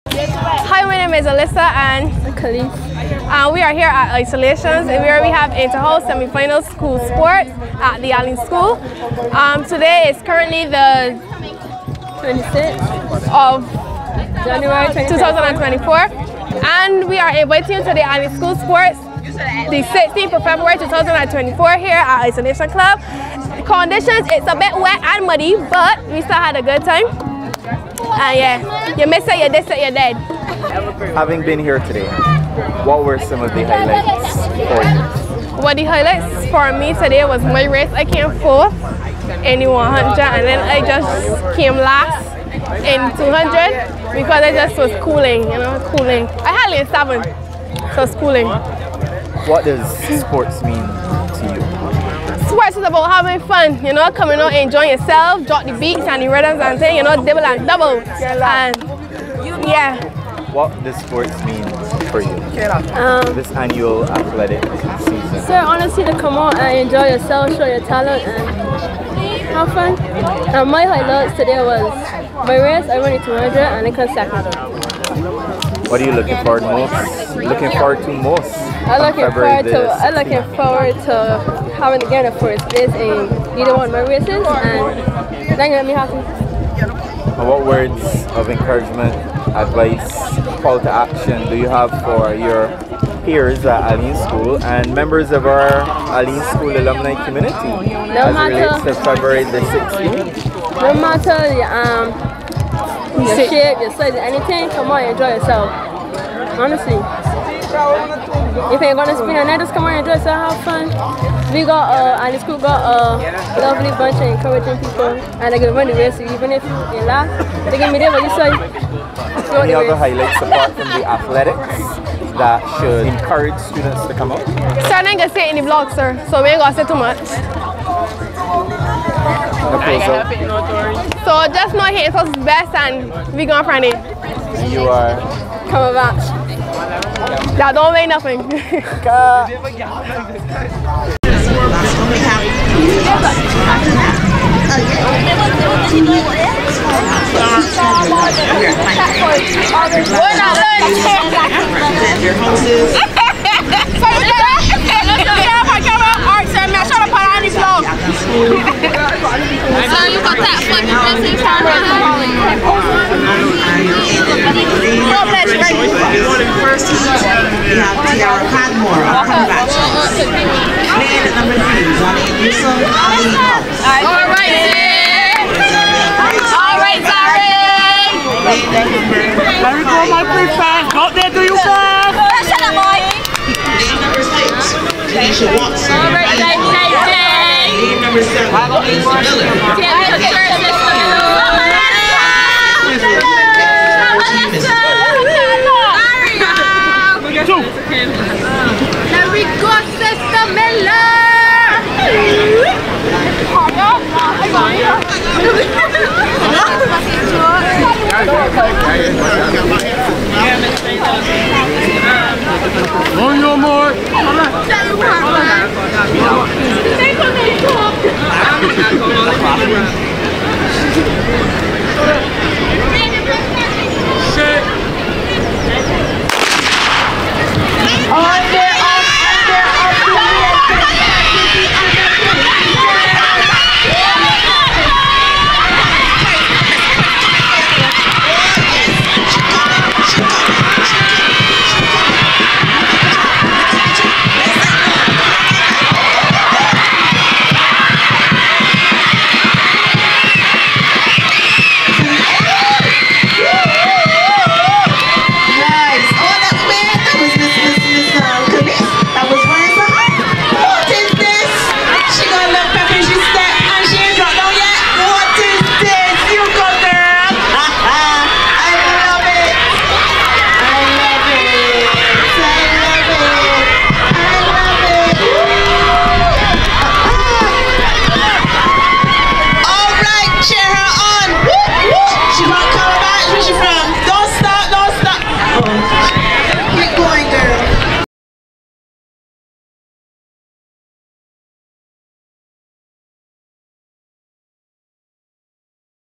ATHLETE INTERVIEW || SEMI-FINAL Inter-house Sports 2024 || Alleyne School [Barbados]
Friday January 26 2024 || Location: Isolation Cavaliers Cricket Playing Field || St. Andrew